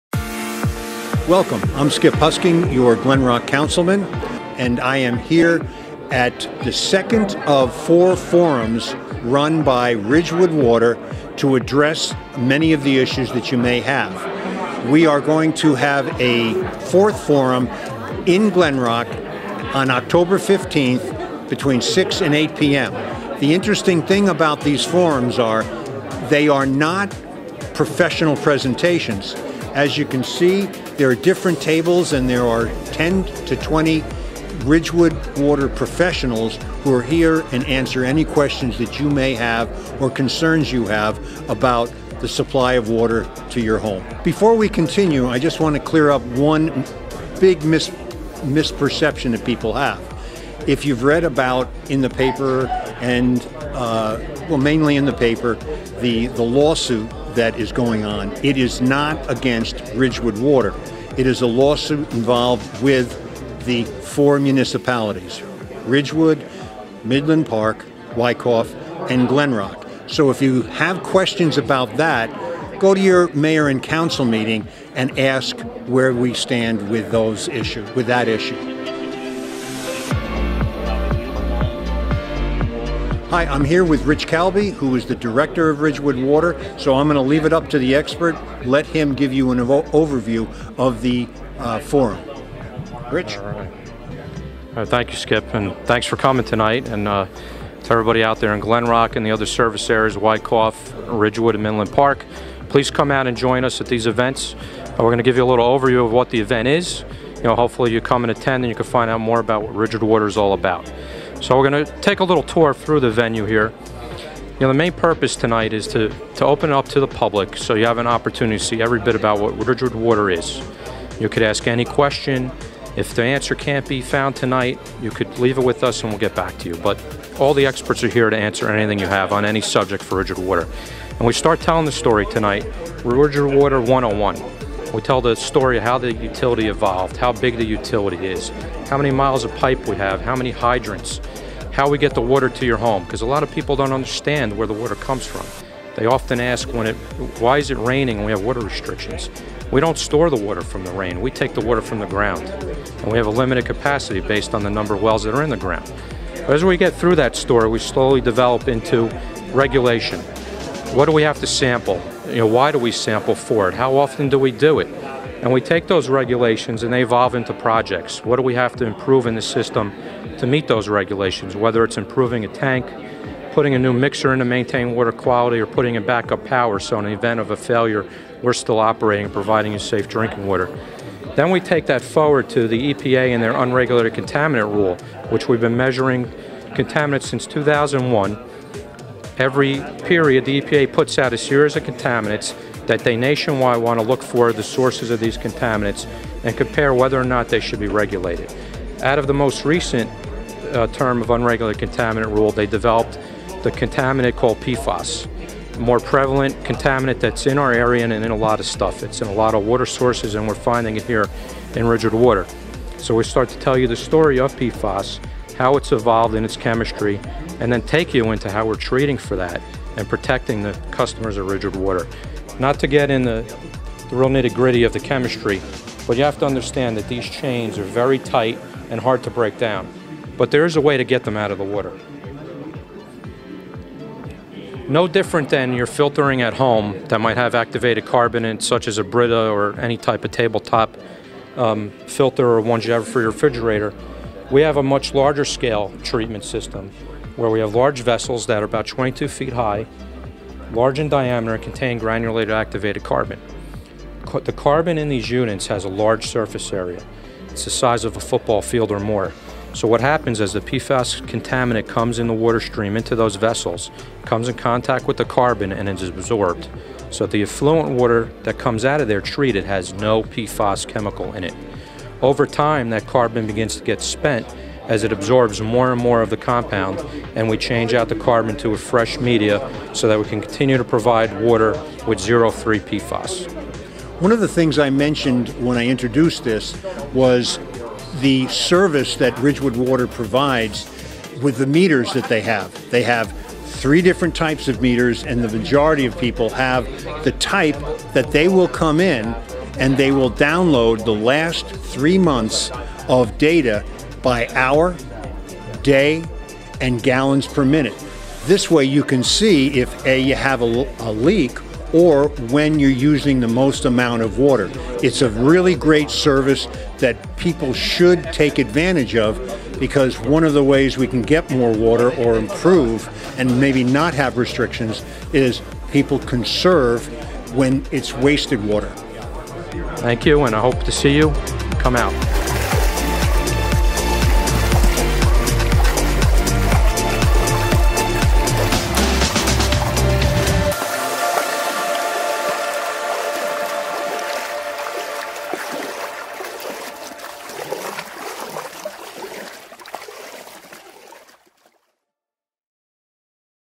Ridgewood Water Forum 10/4/18
The Forum allows residents to talk one on one with RW professionals to express their concerns and answer questions. Some great free services are highlighted.